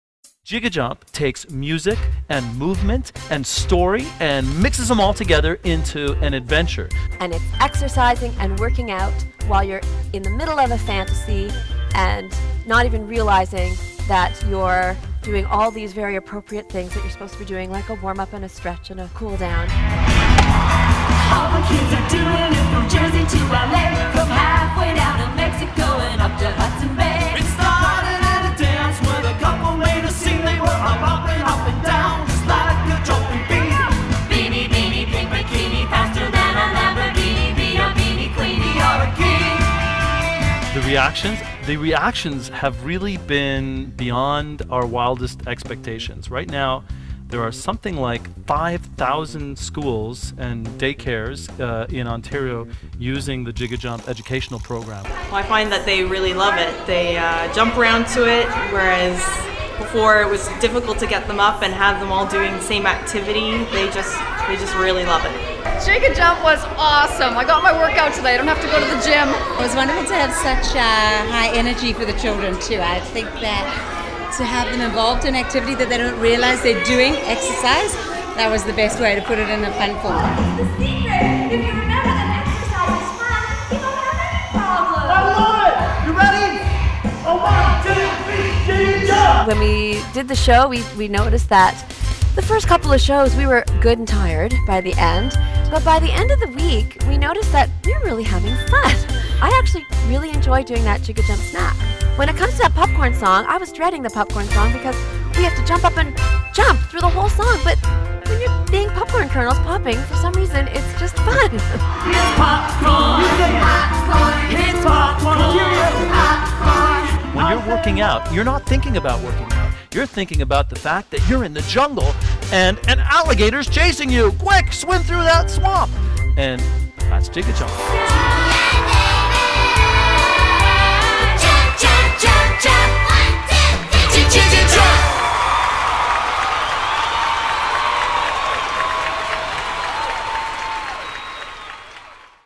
Judy & David's JiggiJump ( formerly JiggaJump )
Clips and info from the first JiggiJump Live Concert tour, starring Canadian sing-along superstars, Judy & David